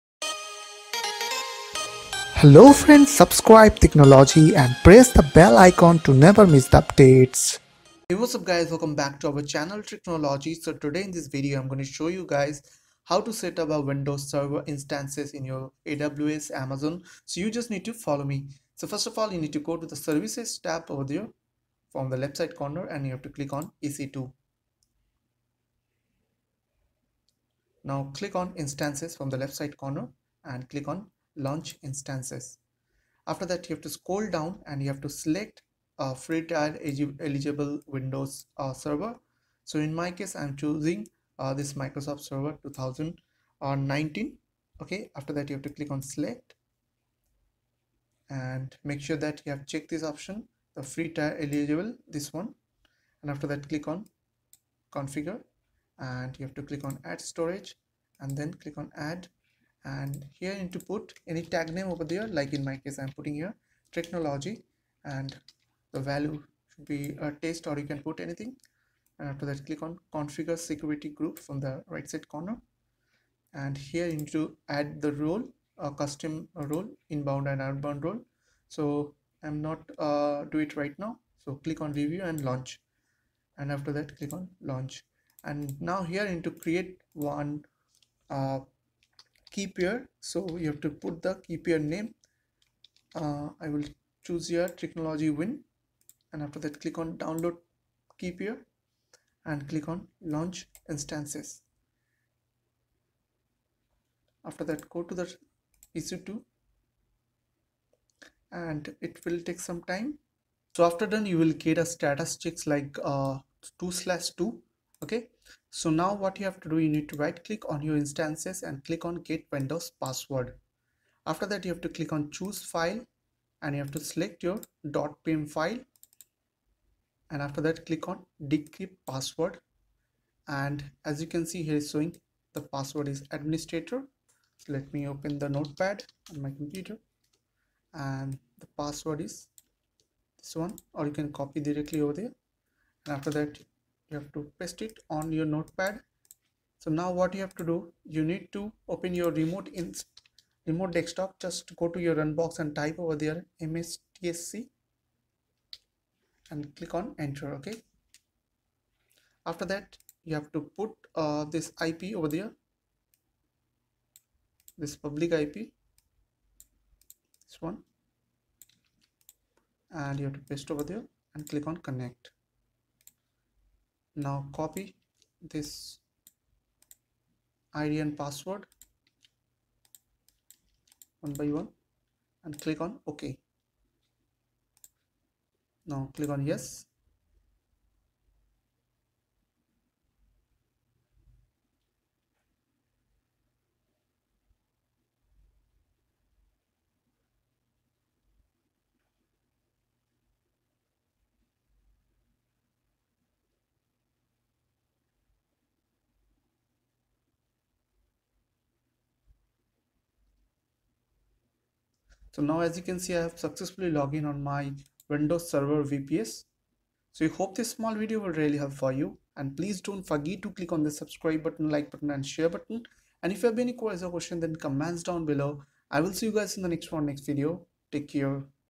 How To Setup Windows Server Instance in AWS EC2 | Amazon Web Service
In this video, I will show you guys how to setup windows server instance in AWS Amazon Ec2.

AWS Amazon

1. Mic
2. Microphone Stand
3. Headphone
4. Phone
5. Strip LED Light
6. Camera